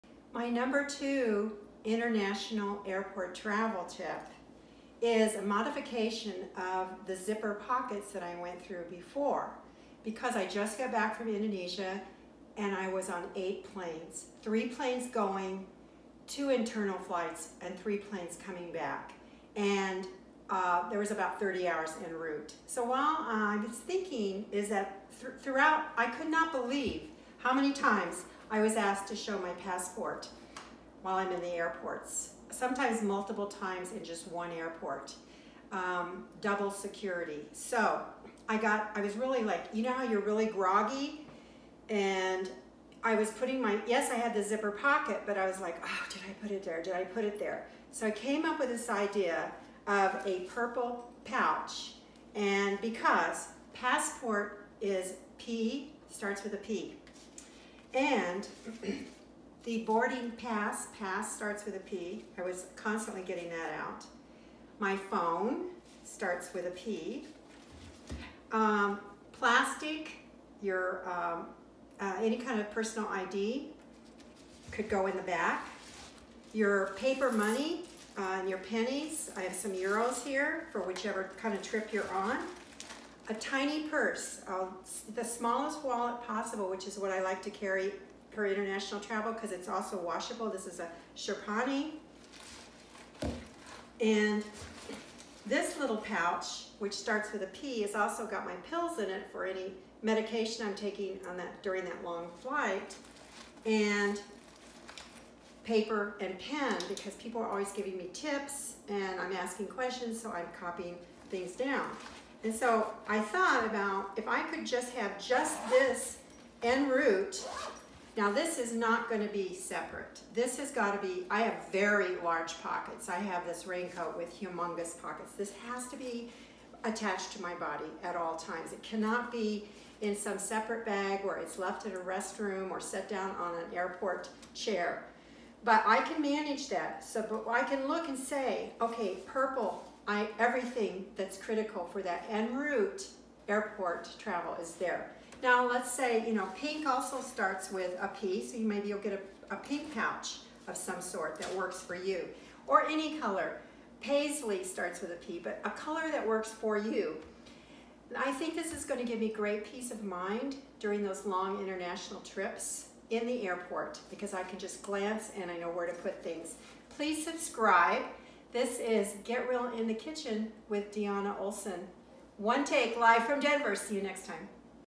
Ep-239 | Don't Lose Your Passport (Purple is the Answer!) International Travel Tip #2023
Ep-239 | Don't Lose Your Passport (Purple is the Answer!) International Travel Tip

After I came up with the idea from the Indonesia trip (7 airports one trip!) to put critical items in a purple pouch, I purchased a purple sling bag (Eddie Bauer has one) and so does Osprey and other brands have purple sling bags.  I put all the critical items in the purple sling from passport to plastic (credit cards) and as mentioned in the video to carry around the airport.  I then used the purple sling for the next trip (to Scandinavia) 4 airports and wandering around airports when my body clock said it's 3 a.m.  It worked great to put my critical items in the airport in the color coded purple sling to boldly remind myself to keep everything together and zipped!

𝐖𝐚𝐧𝐭 𝐭𝐨 𝐬𝐞𝐞 𝐦𝐨𝐫𝐞 𝐜𝐨𝐧𝐭𝐞𝐧𝐭 𝐥𝐢𝐤𝐞 𝐭𝐡𝐢𝐬?
🔗 Share the video with anyone